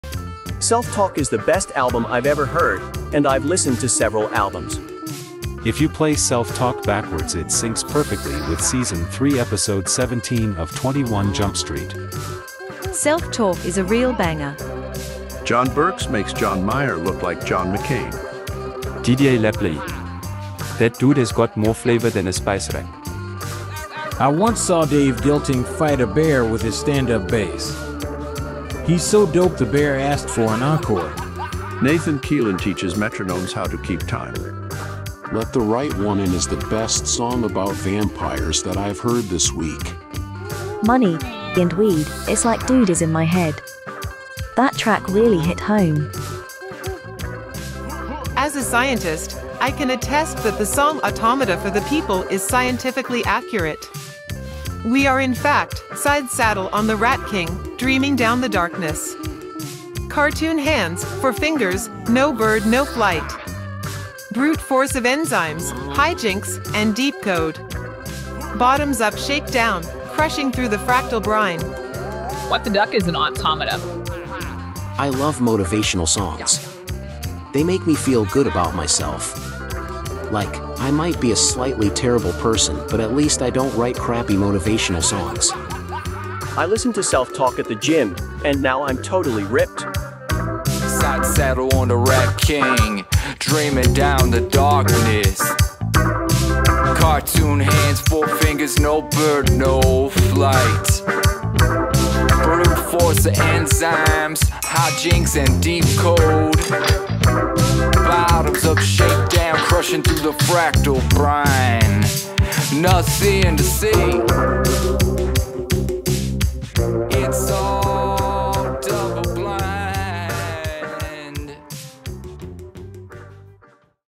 The People Have Spoken
Here are some 'man on the street' reviews of the new album. I think they like it!